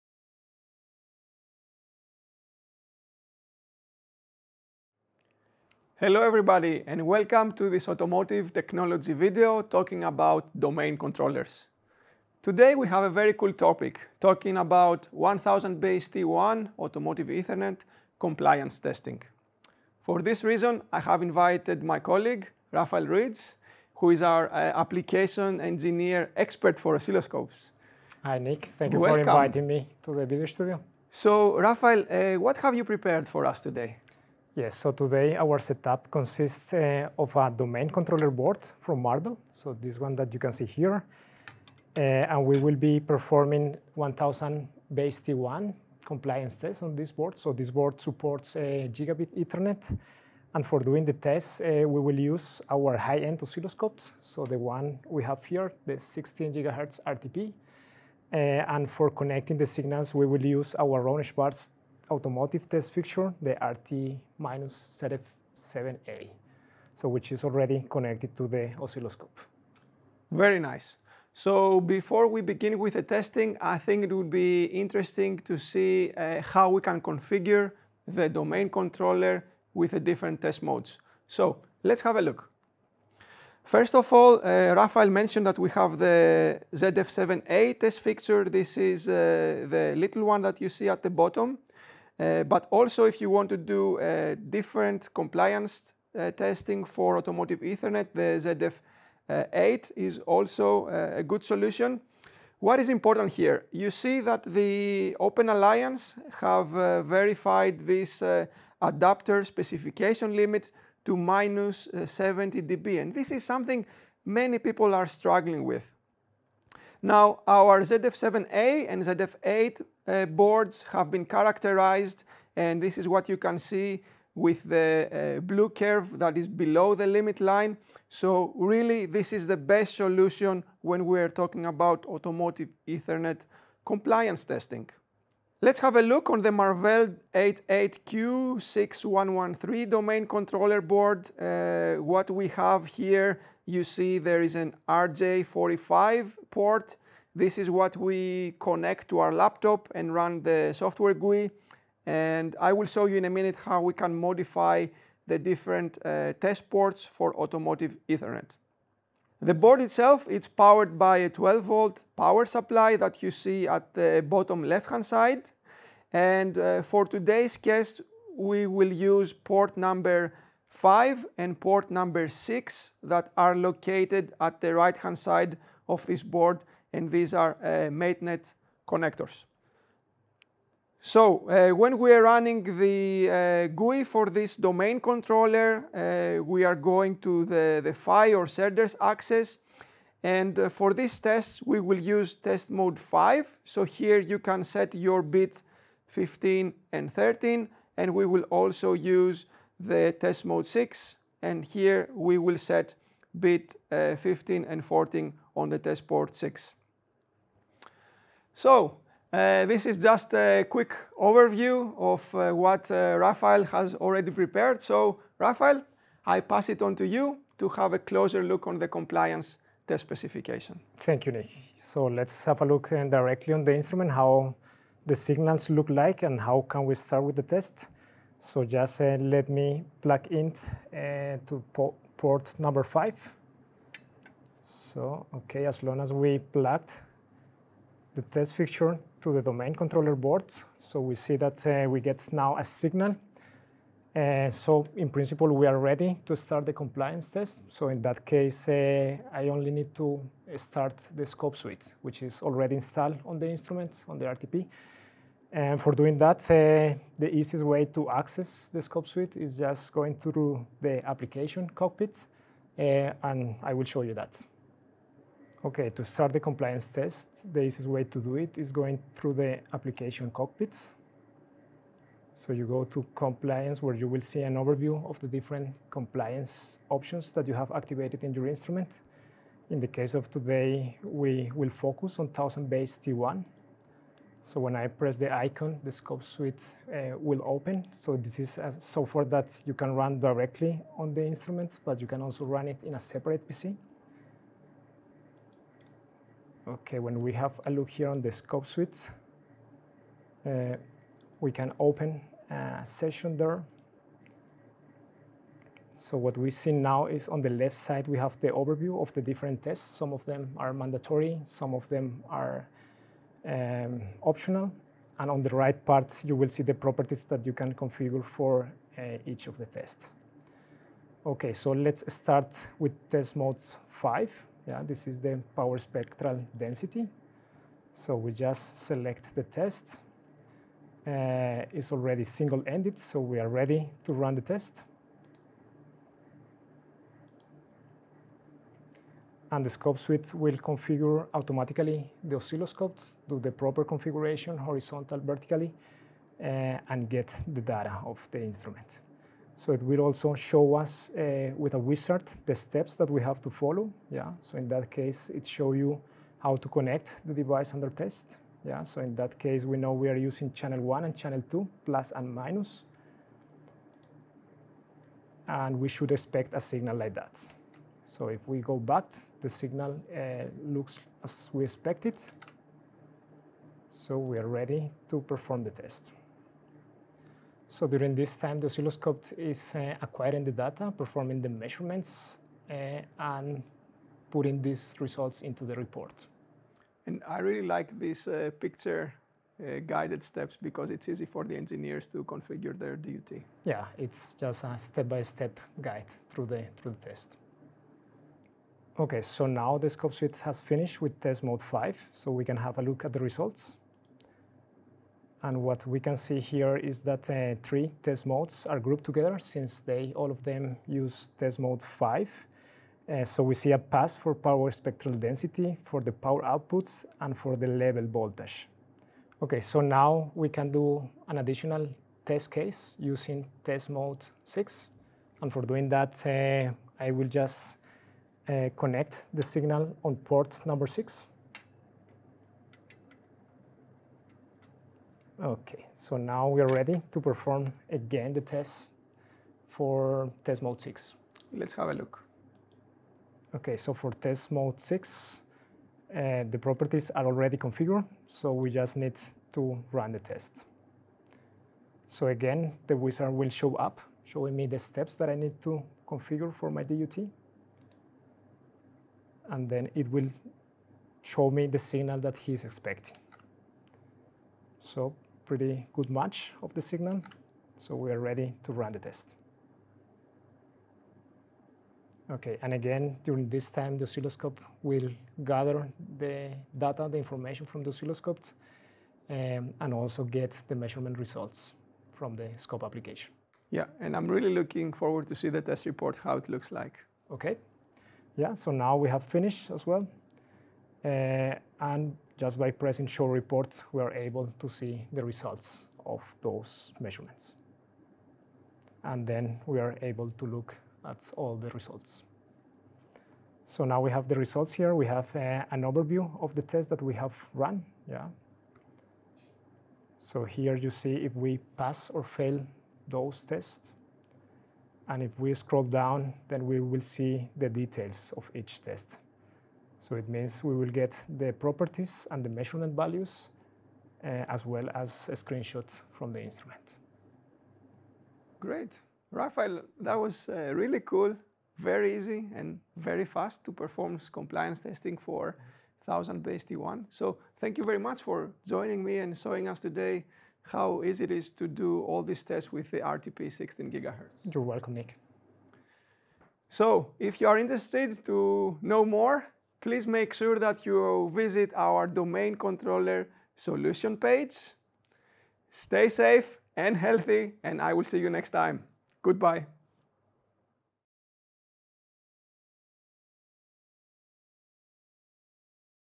Application video: 100BASE-T1 automotive Ethernet compliance testing
Learn more about the test techniques for 1000BASE-T1 compliance testing on a domain controller.